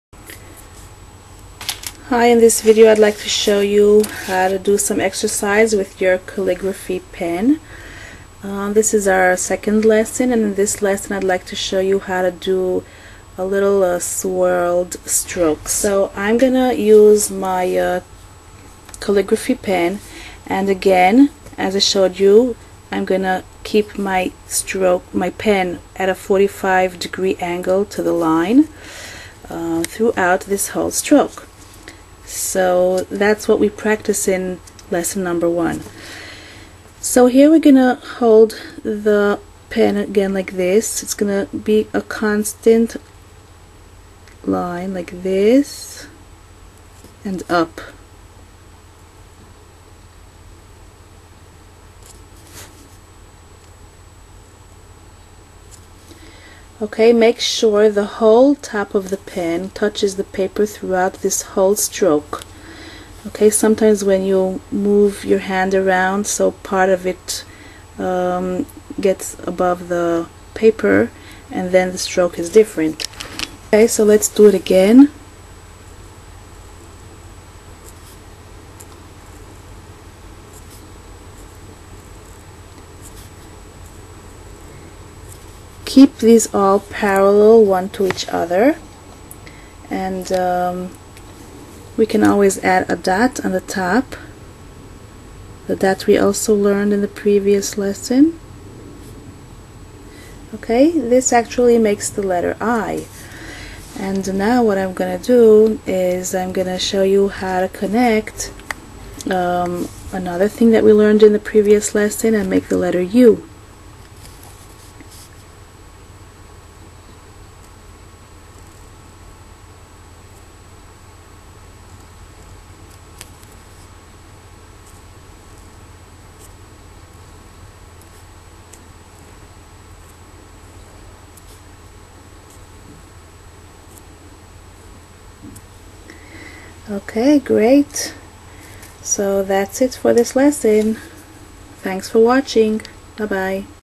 Calligraphy Writing - Practice Basic Swirled Strokes Part 2
For more info on this calligraphy pen

Calligraphy basic training -practice  calligraphy pen strokes
how to use calligraphy pen with swirled strokes for swirled letters